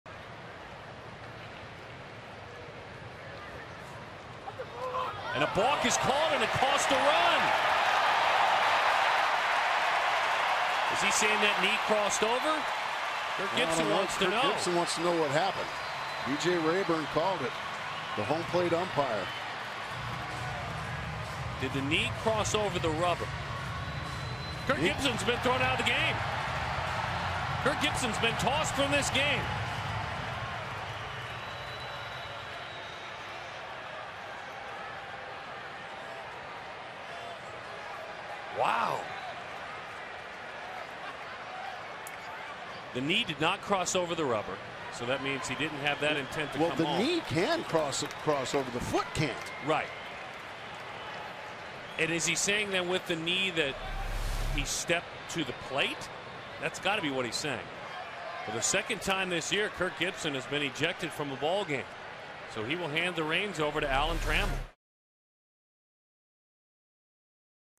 2011/05/29 Gibson's ejection
ARI@HOU: Gibson is ejected for arguing a balk